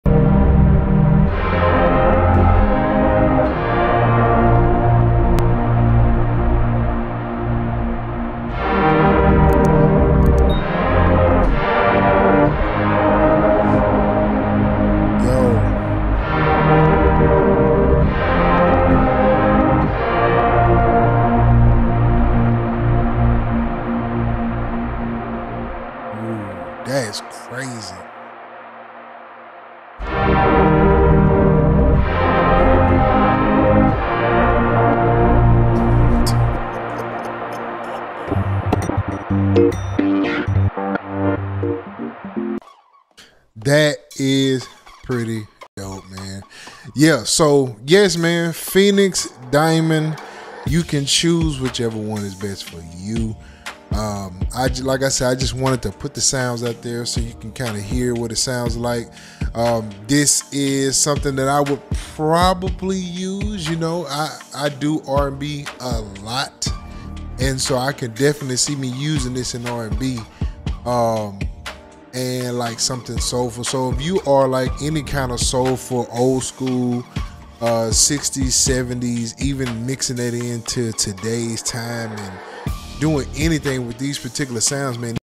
Soulful R&B Music Production: Creating Vintage Sounds for a Modern Twist
Discover how to incorporate old-school 60s and 70s vibes into your soulful R&B tracks. Join us in this tutorial as we explore the perfect sounds and techniques to give your music a timeless feel.